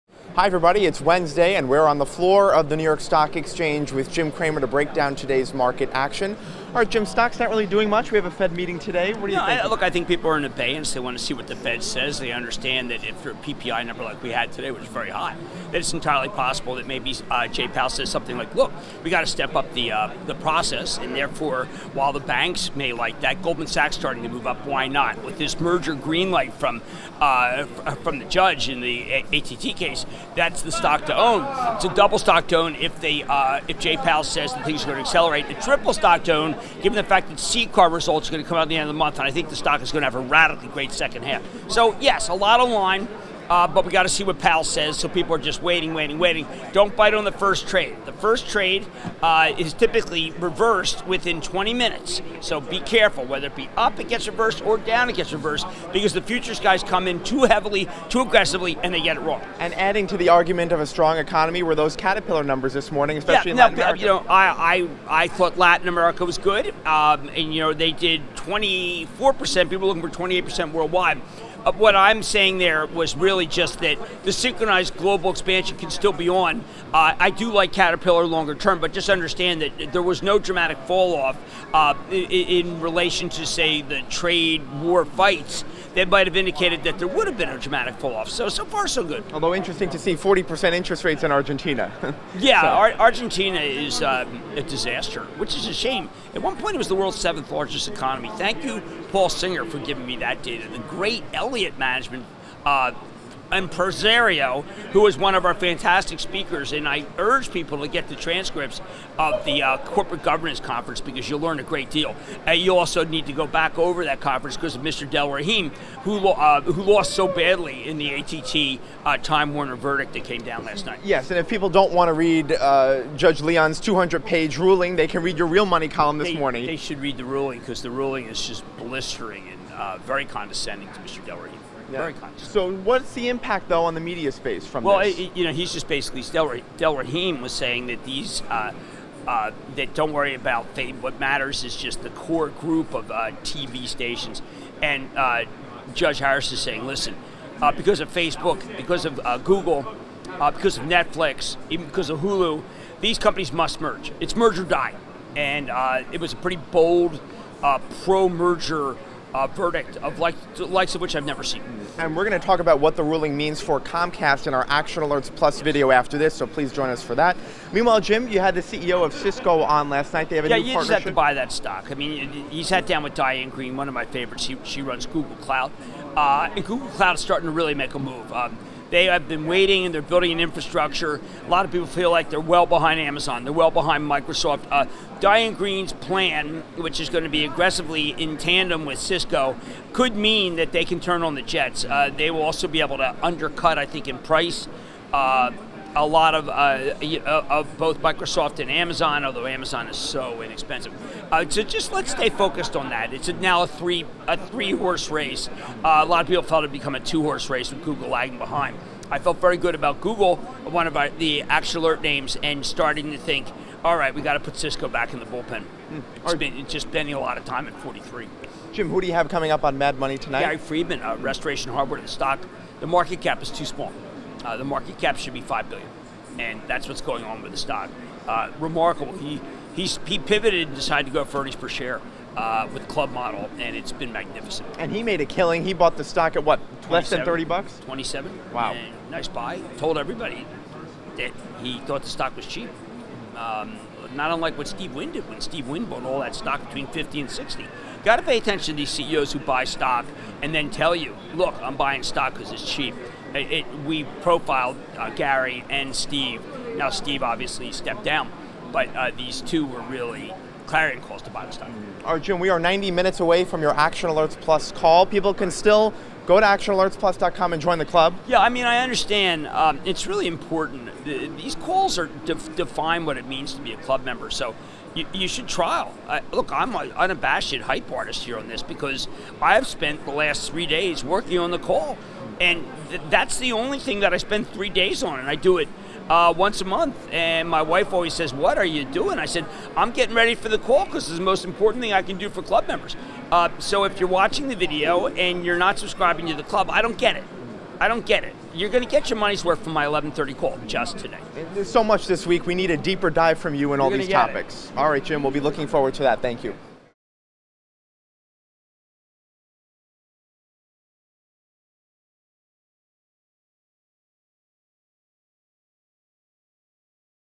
Jim Cramer on AT&T, Time Warner, Caterpillar and Cisco
TheStreet's founder and Action Alerts PLUS Portfolio Manager Jim Cramer weighs in on Wednesday's trending stocks from the floor of the New York Stock Exchange including AT&T, Time Warner, Caterpillar, and Cisco.

Sign up for ActionAlertsPlus.com today for exclusive insight into Jim Cramer’s charitable portfolio